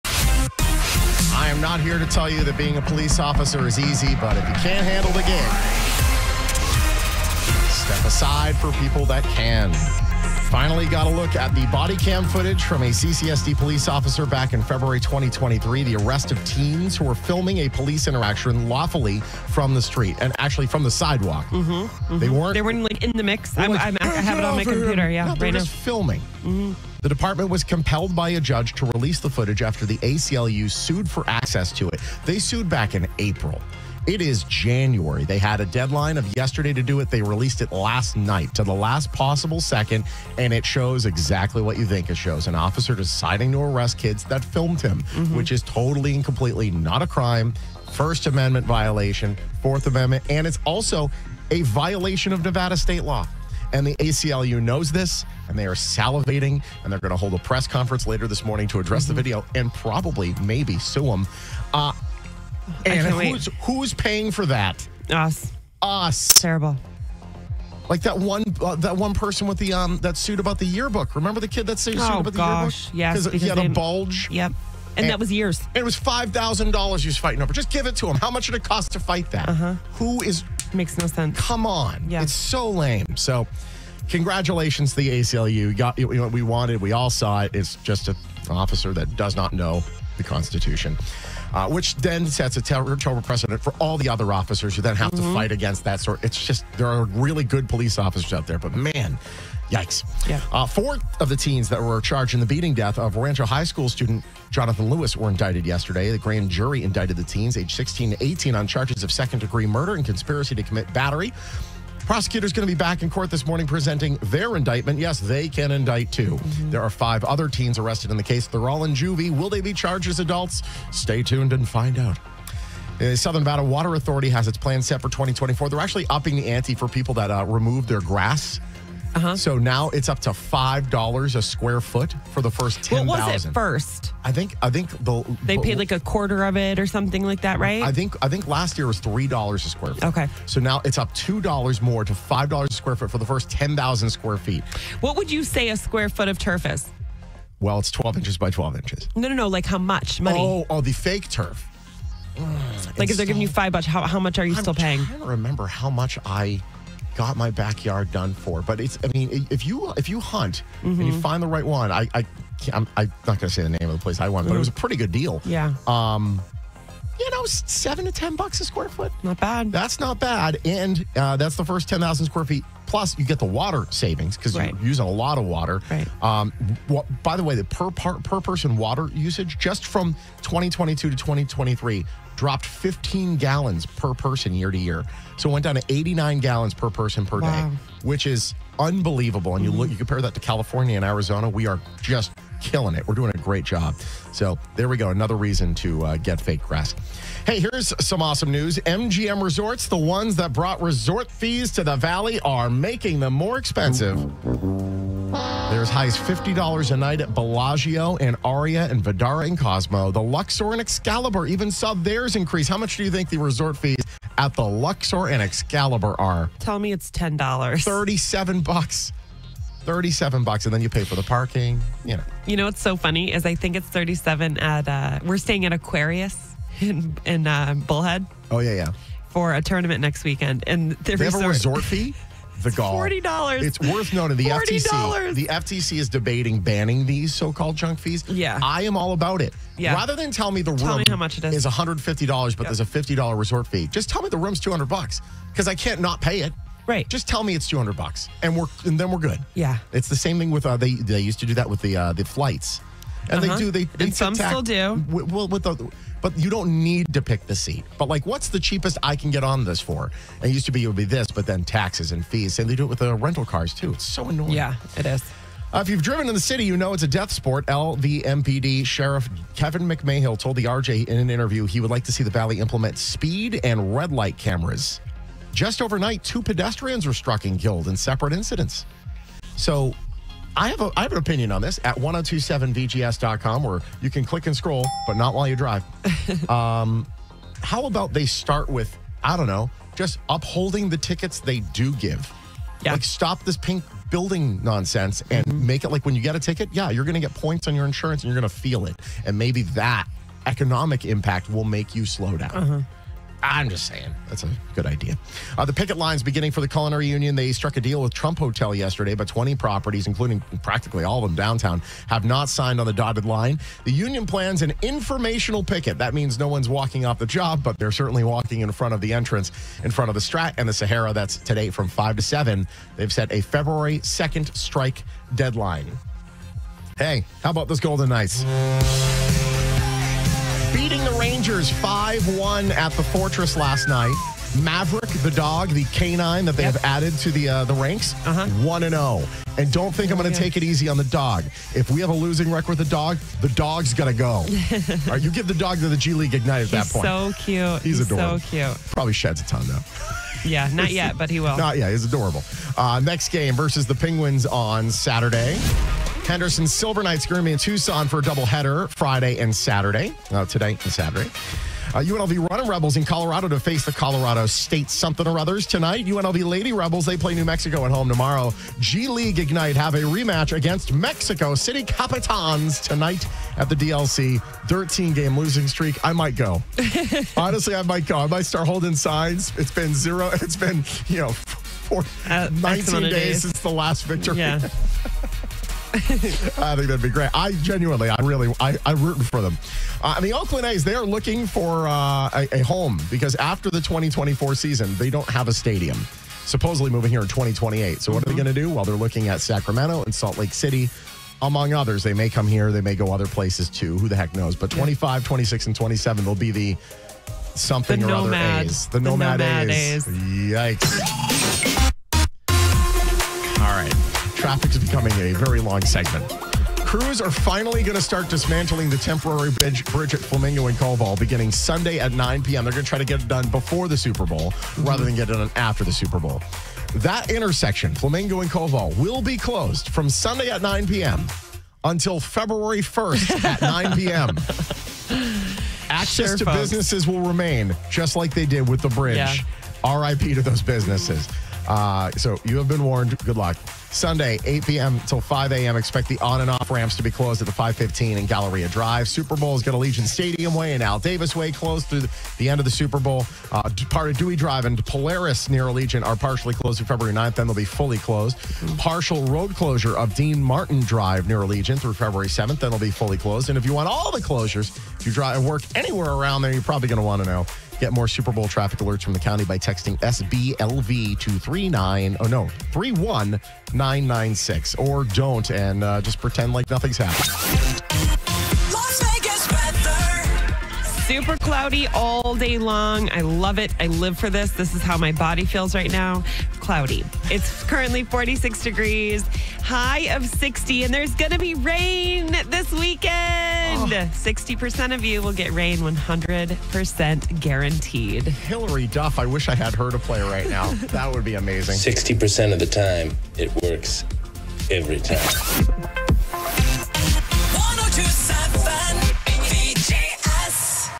Need To Know: CCSD Finally Release Bodycam Footage Of Durango High Arrests
It took 11 months, but CCSD PD finally released bodycam footage of the unconstitutional arrest of teens at Durango High School that filmed them arresting another teen. Now the ACLU is planning a press conference and this could get expensive for the Clark County School District (and taxpayers).

Plus MGM Resorts is back at it raising Resort Fees.

📷 Clark County School District Police Department